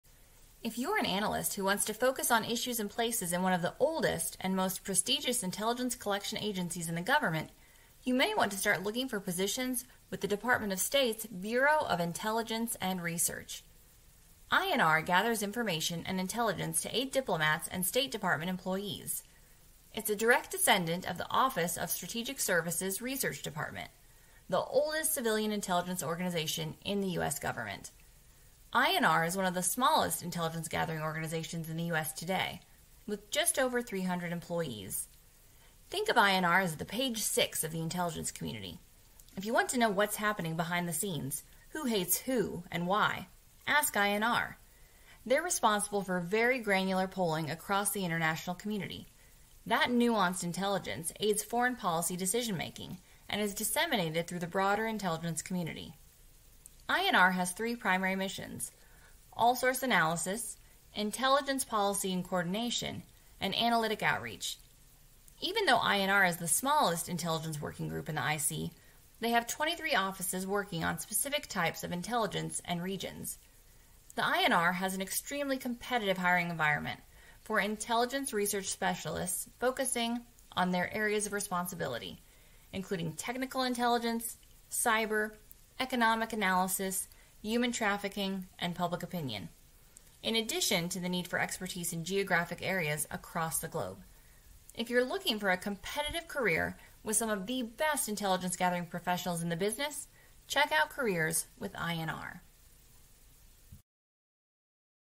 Learn all about the Smallest Organization in the Intelligence Community
If you’re an analyst who wants to focus on issues and places in one of the oldest and most prestigious intelligence collections in the government, you may want to start looking for positions with the Department of State’s Bureau of Intelligence and Research.